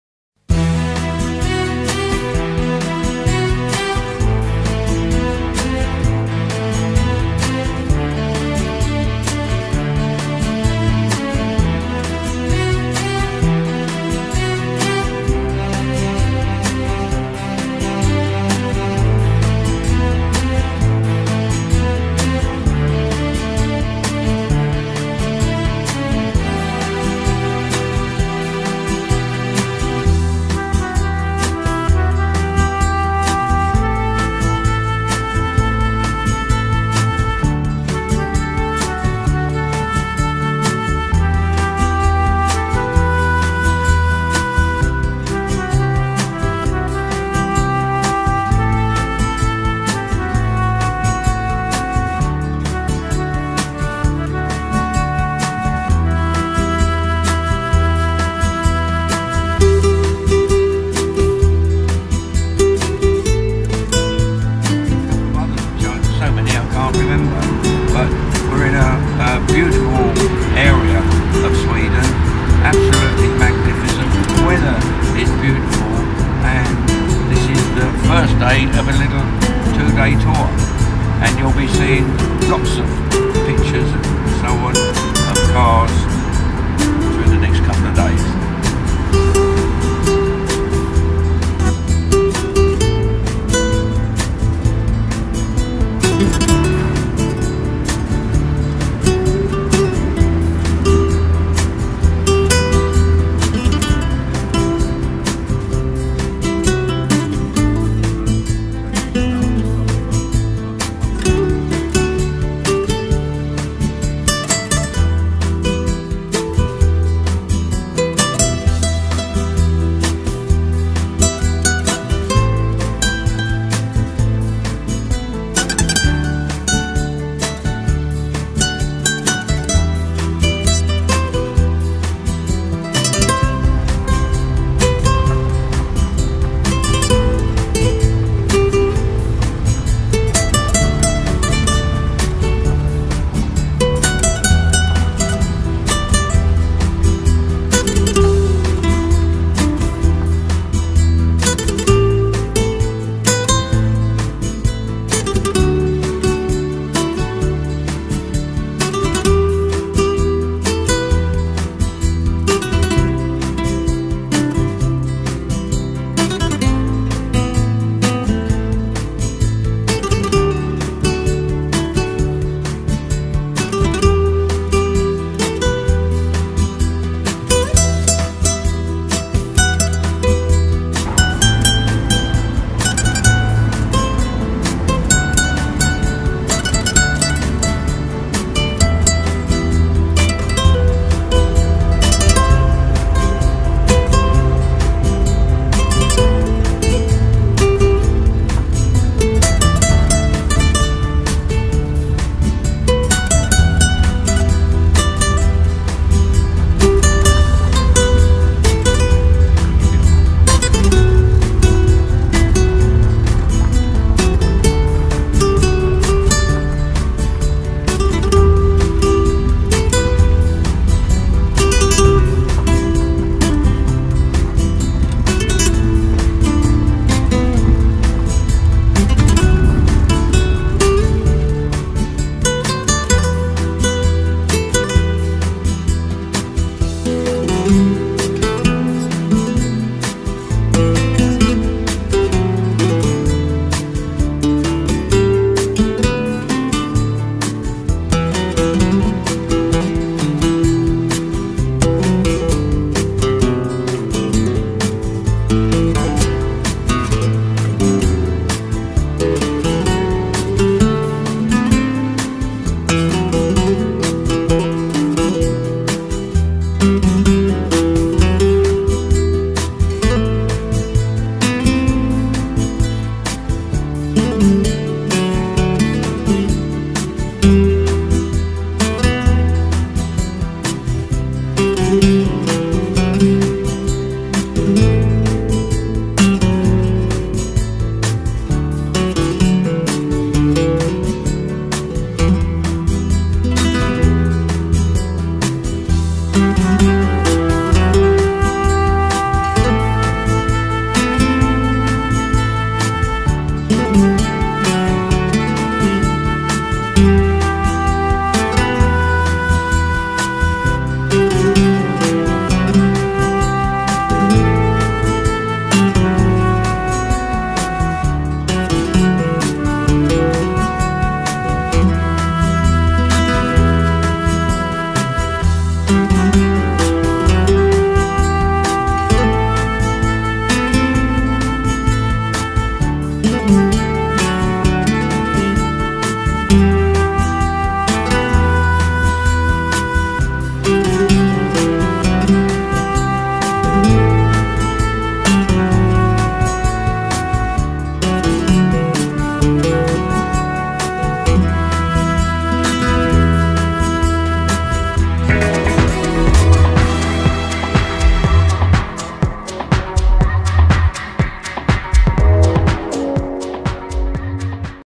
Memories of Sweden
A trip in an old timer car